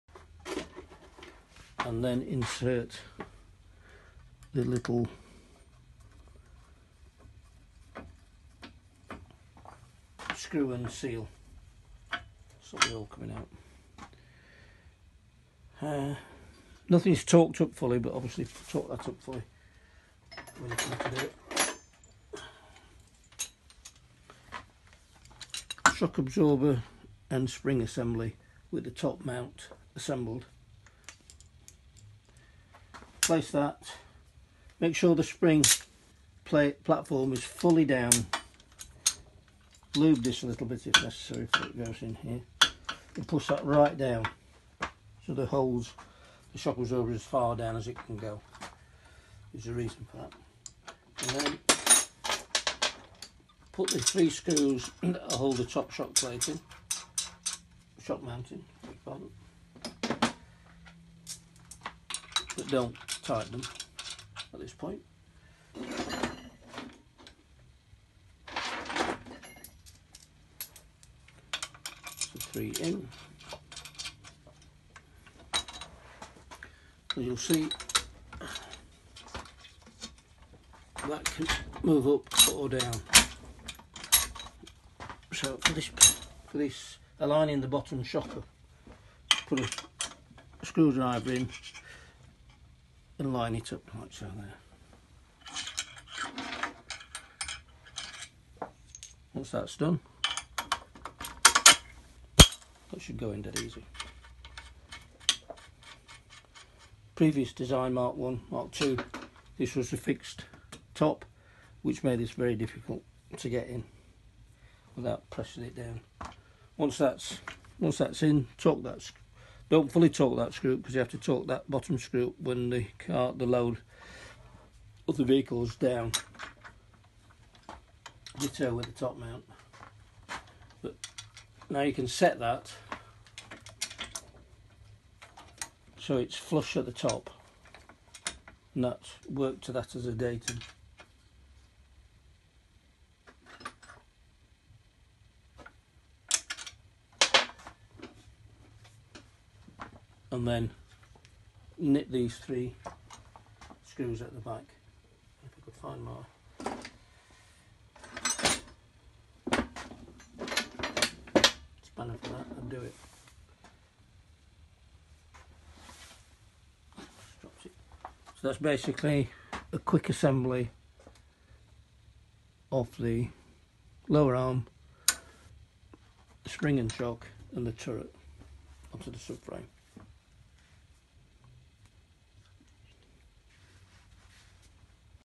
Mike Satur MGF Hydragas to Coil Conversion kit demonstration part 2 - FCCKit
Mike Satur MGF coil conversion kit demo part 2 of 2